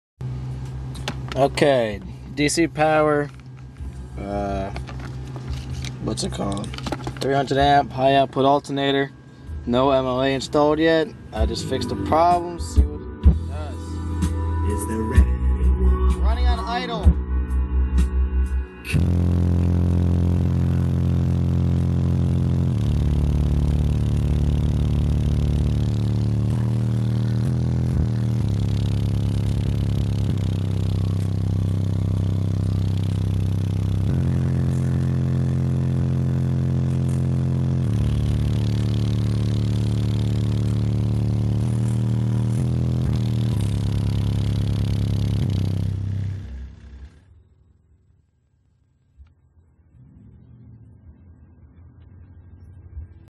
DC power alt at idle, subwoofers
4 15 inch T2s powered by an EarthQuake D3 on my new DC Power 300A alt at idle